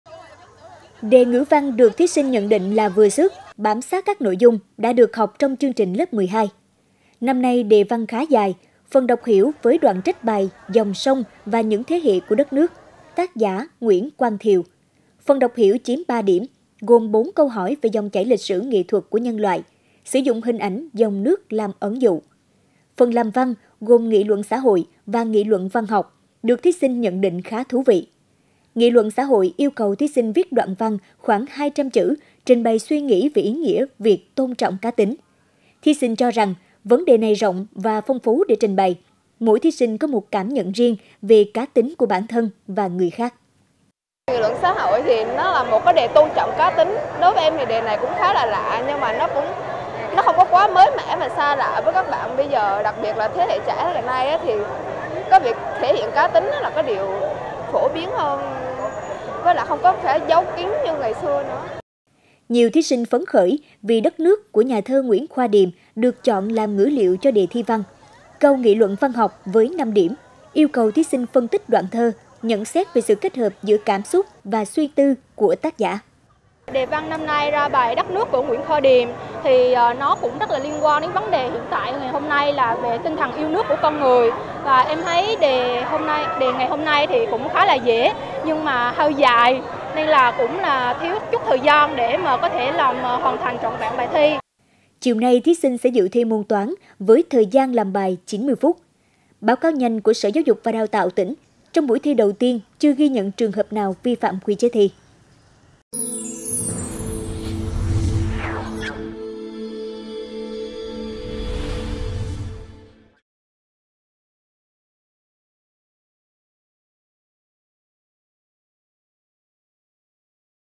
Gần 14 ngàn thí sinh Quảng Ngãi bước vào kỳ thi tốt nghiệp THPT 2024 | PTQ
Gần 14 ngàn thí sinh Quảng Ngãi bước vào kỳ thi tốt nghiệp THPT 2024

Sáng nay(27/6), cùng với hơn 01 triệu thí sinh của cả nước, gần 14 ngàn thí sinh của Quảng Ngãi đã bước vào kỳ thi tốt nghiệp THPT năm 2024. Môn thi đầu tiên là Ngữ văn, thời gian làm bài là 120 phút.